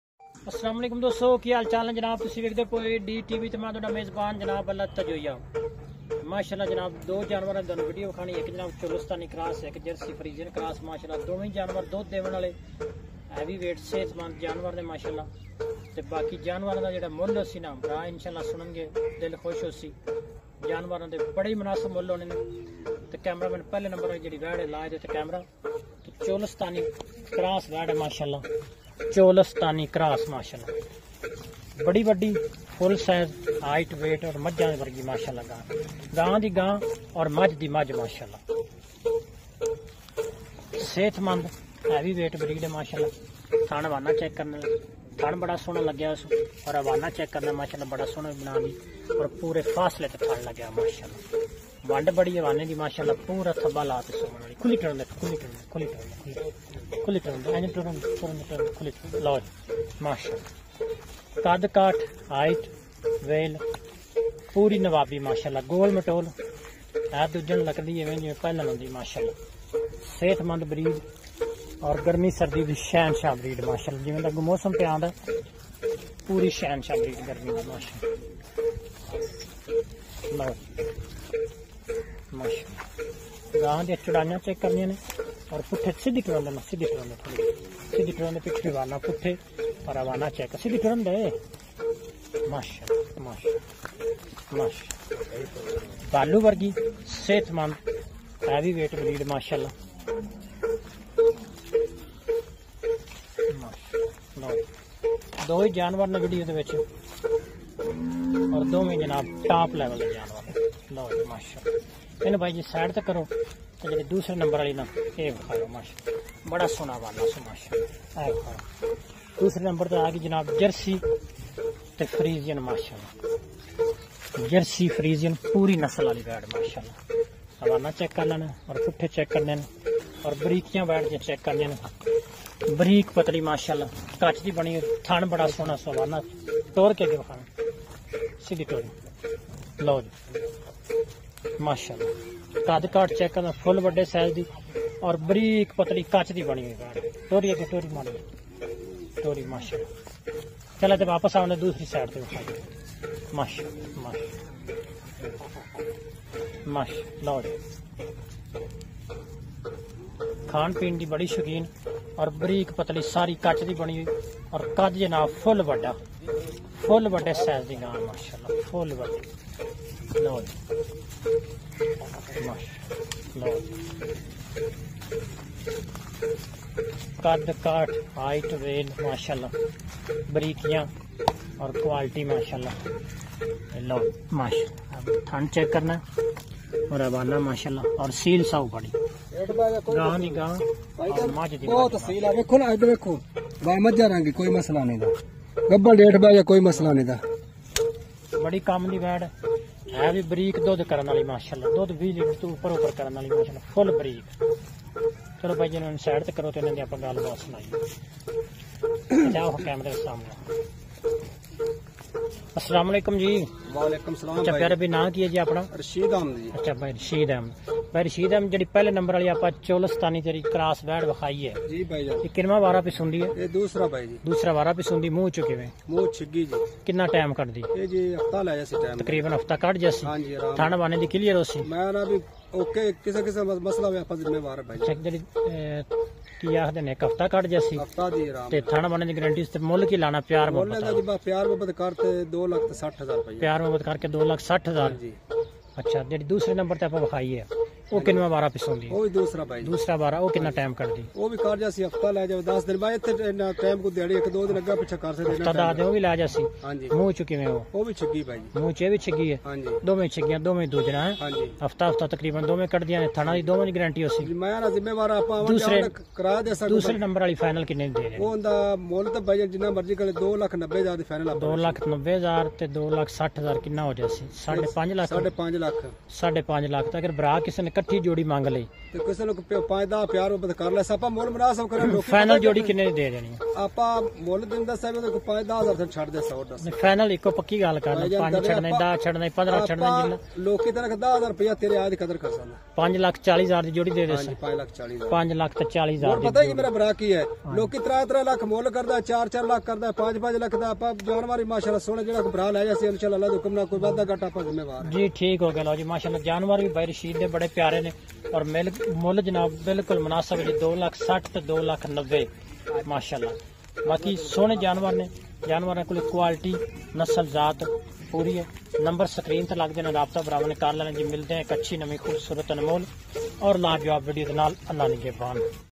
2 cholstani cross cow for sale 3 May 2023 in the tehsil depalpur district okara Punjab Pakistan
2 cholstani cross cow for sale in the tehsil depalpur district okara Punjab Pakistan on YouTube Allah ditta joyia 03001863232